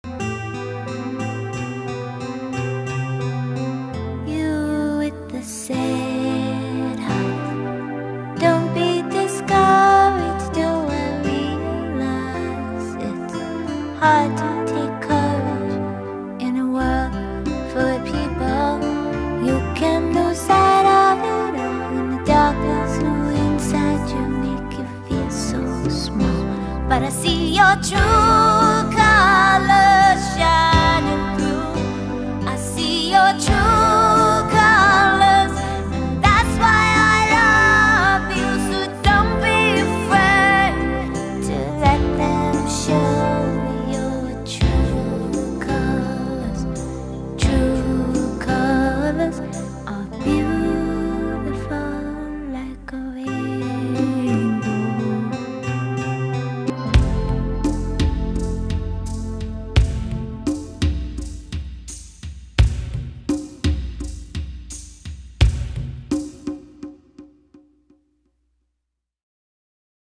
Your True Colors--Please Foster these Adorable Cats
Ollie's Place, 430 E 9th St, NYC needs three-month foster homes for these sweet kitties by February 1st.